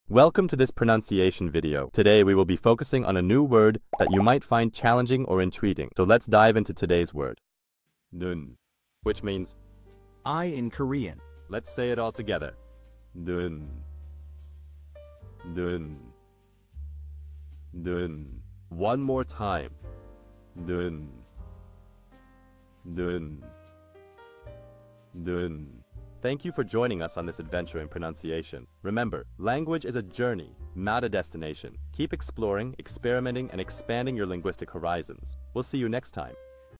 How to pronounce nun | 눈 (eye in Korean)
Master the Pronunciation of 'nun | 눈' - which means : eye in Korean 📝 with @PronunciationKorean 🎓🗣️ - [Your Guide to Korean]

Learn to pronounce 'nun' perfectly in Korean with our easy and engaging tutorial by @PronunciationKorean! 📖📽️ In this comprehensive video, we provide you with native speaker examples 🗣️👂 and phonetic breakdowns 🎵 to ensure you get the pronunciation just right.